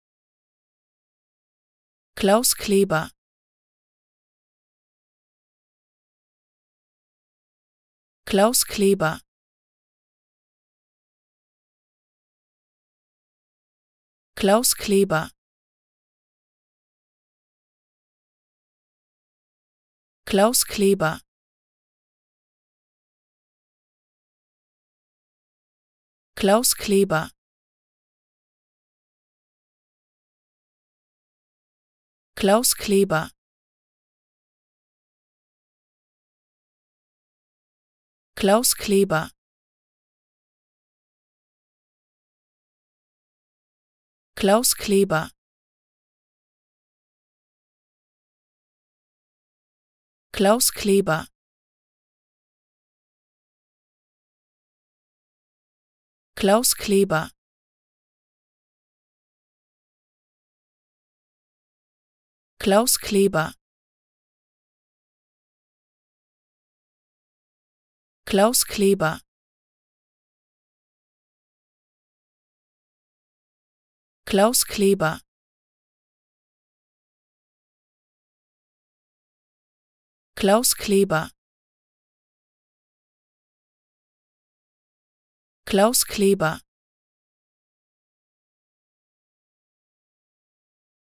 How to Pronounce ''Claus Kleber'' Correctly in German
Learn how to say ''Claus Kleber'' in German with this short pronunciation tutorial.